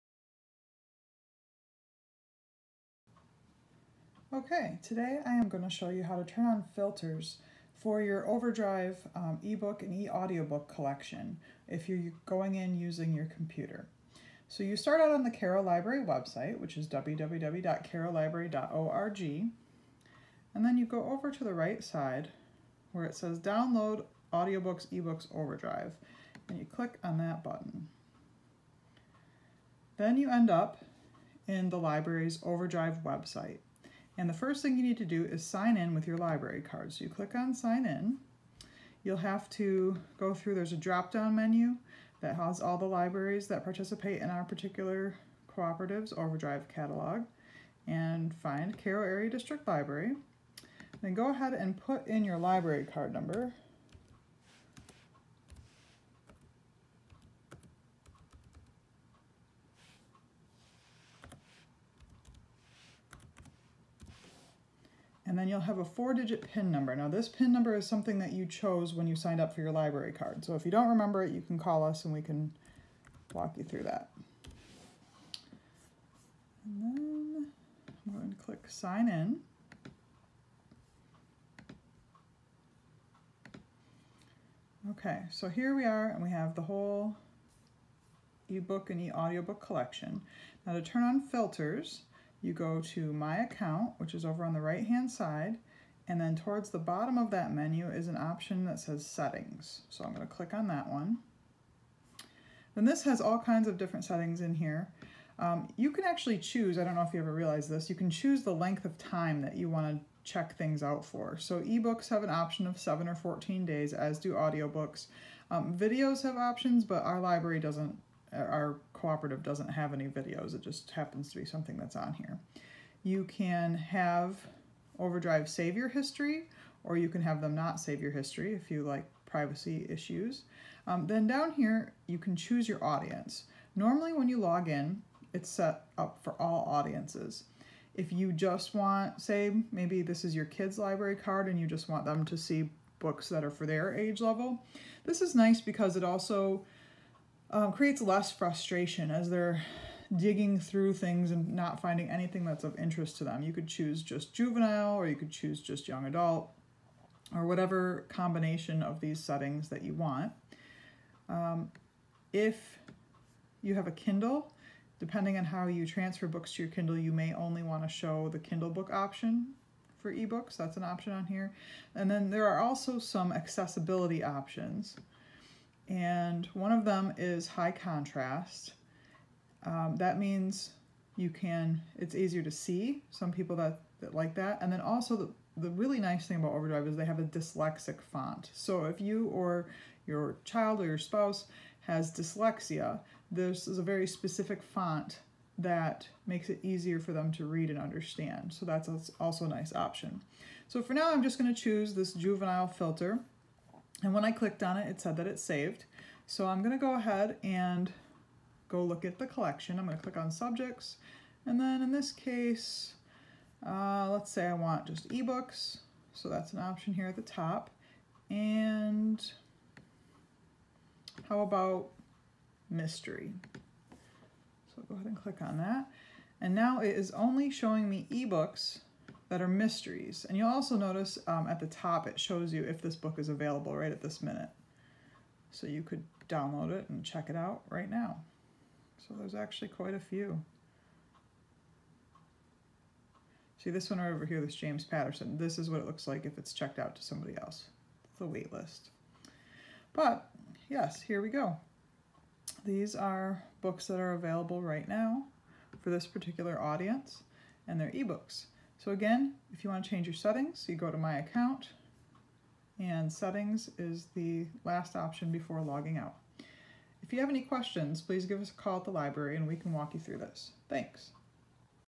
How to Change Settings in Overdrive Using Your PC
Learn to change your settings in Overdrive if you are accessing it with a PC